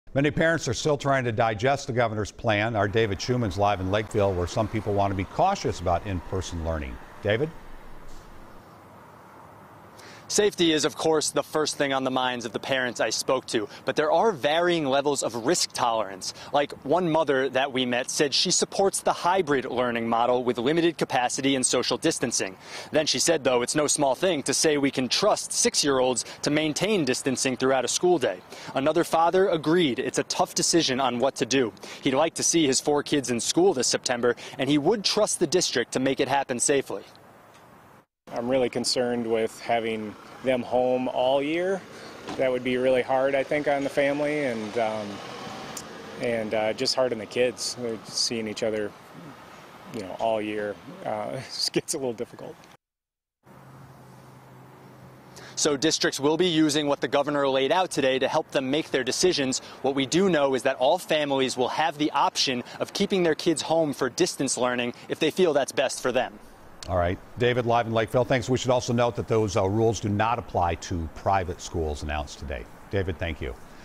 Parents Left With Questions Over School Plan
Gov. Tim Walz has made his long awaited announcement on schools, but while it explains some things, it doesn't answer definitively the biggest question parents have been asking, David Schuman reports (1:13). WCCO 4 News At 6 - July 30, 2020